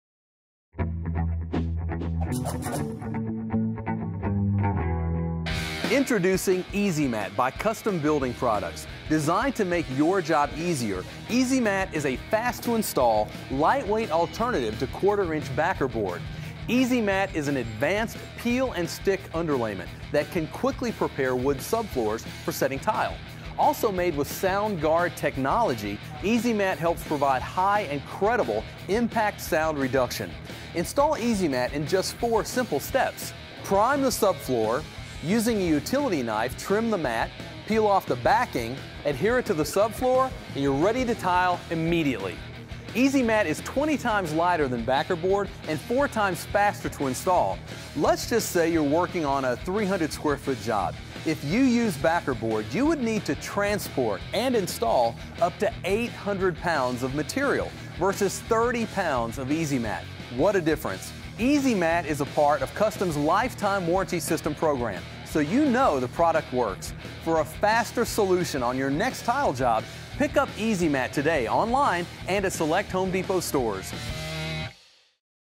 EasyMat Tile and Stone Underlayment for Pros - The Home Depot
EasyMat is a versatile mat underlayment that is 20X lighter than backerboard and 4X faster to install. Used for interior and exterior applications, EasyMat creates a bonded crack prevention system and also offers impact sound reduction.